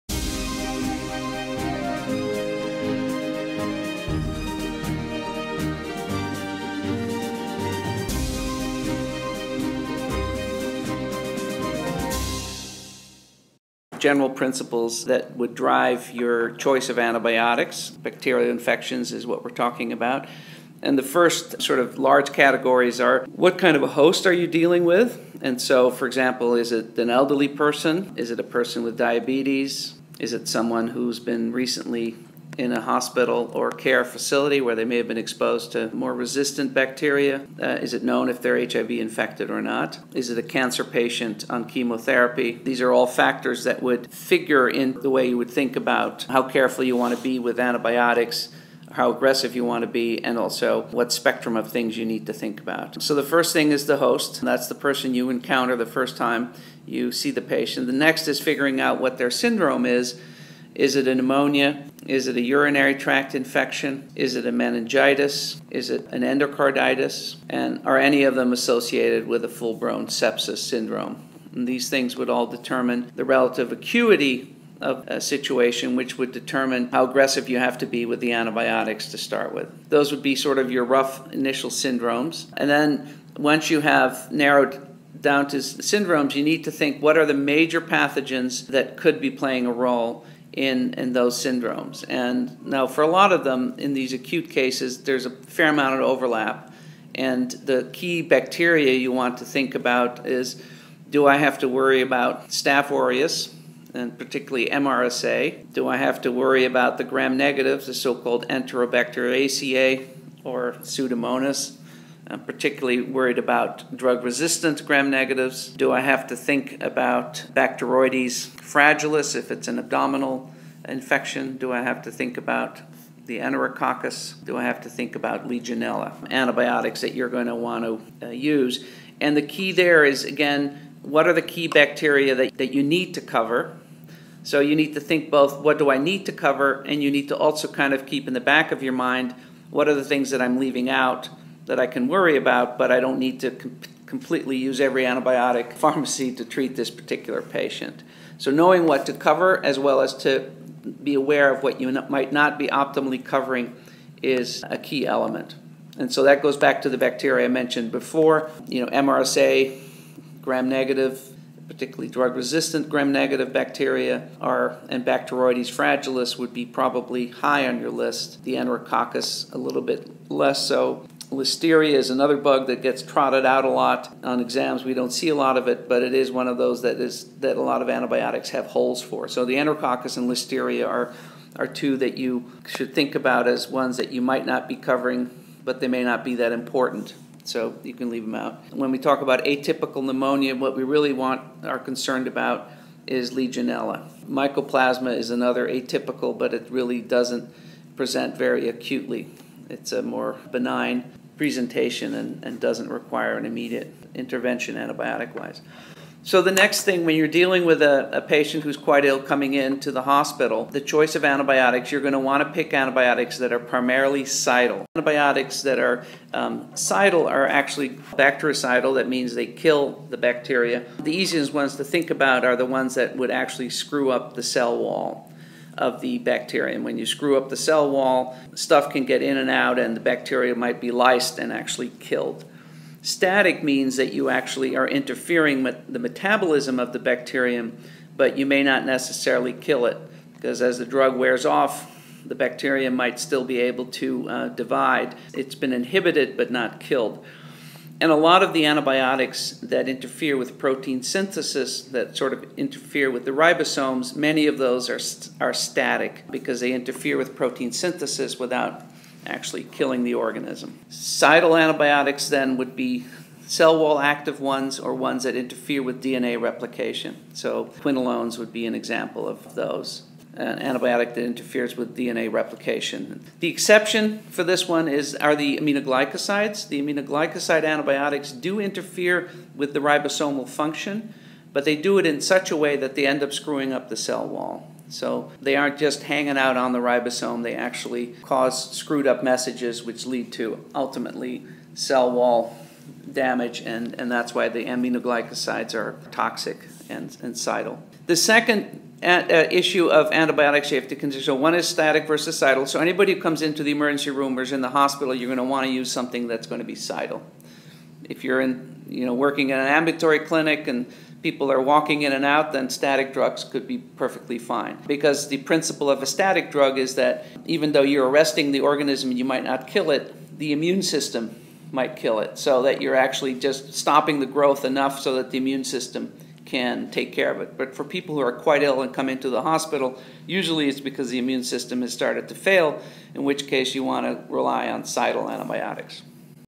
4 Basic Steps in Choosing Antibiotics
Dr. Henry Boom provides an overview of the host, syndrome, pathogen, and drugs in relation to antibiotic selection. He also discusses bactericidal vs bacteriostatic drugs.